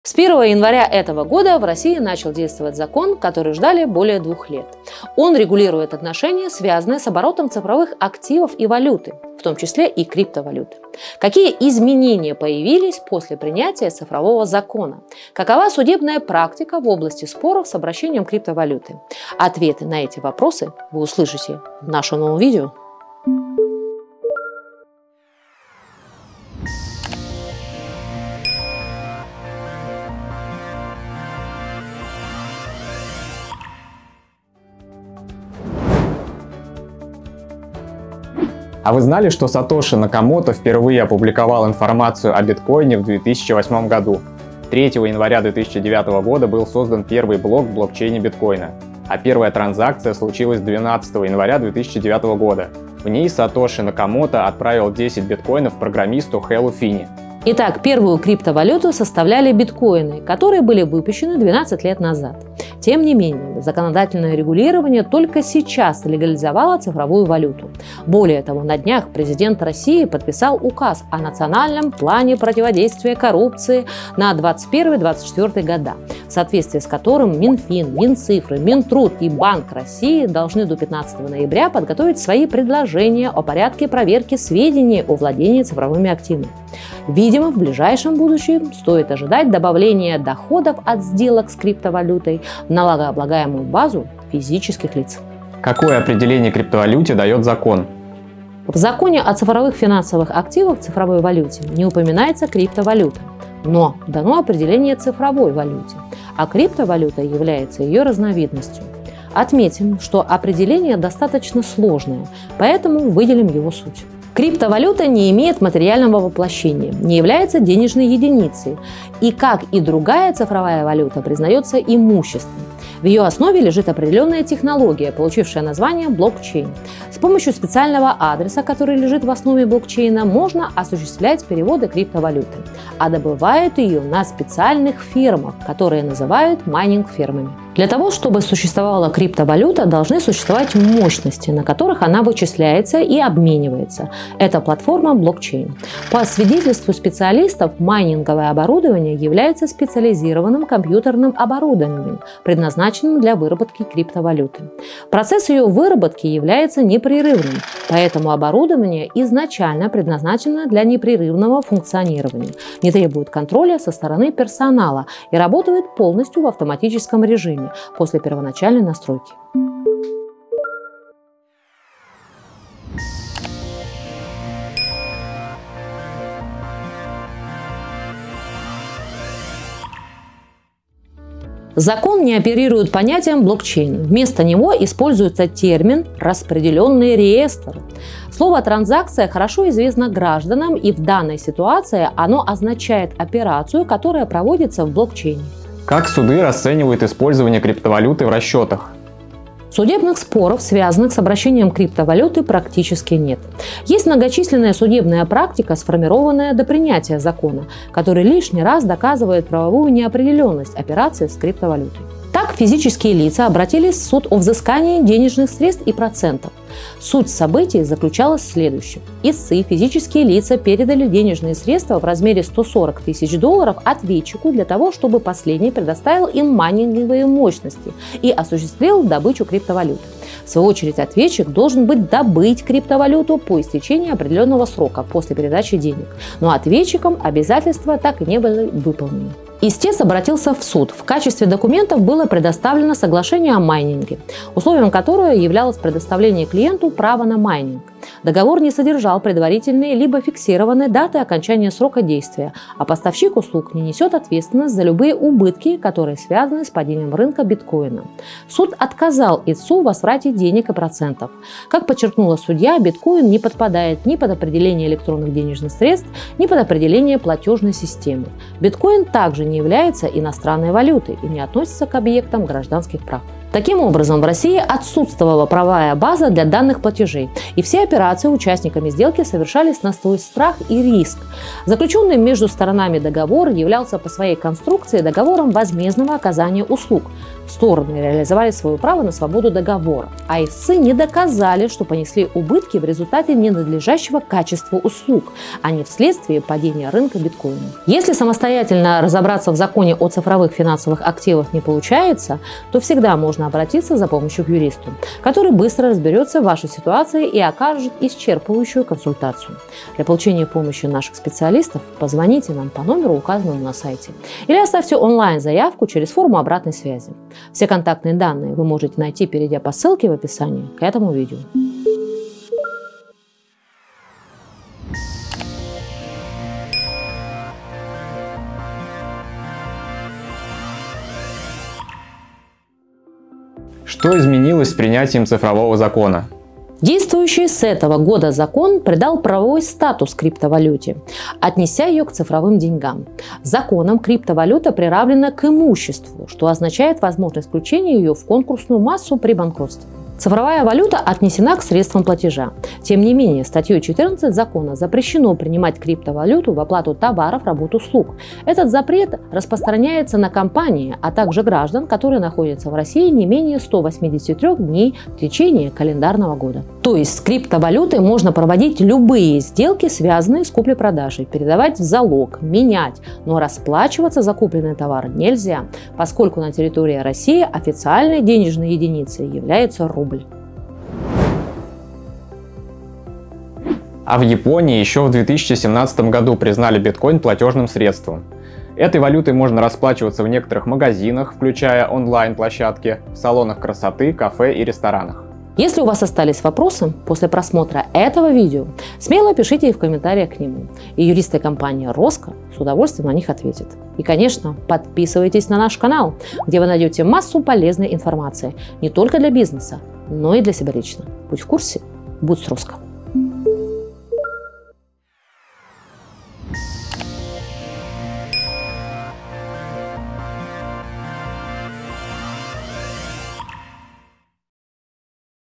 Законодательное регулирование криптовалюты
С 1 января 2021 г. в России действует Закон, который регулирует отношения, связанные с оборотом цифровых активов и валюты, в т.ч. и криптовалюты. Выхода Закона, который бы регулировал правовое положение криптовалюты, ждали более двух лет.

1. Что такое криптовалюта?
2. Как суды расценивают использование криптовалюты в расчетах?
3. Что изменилось с принятием «цифрового» Закона?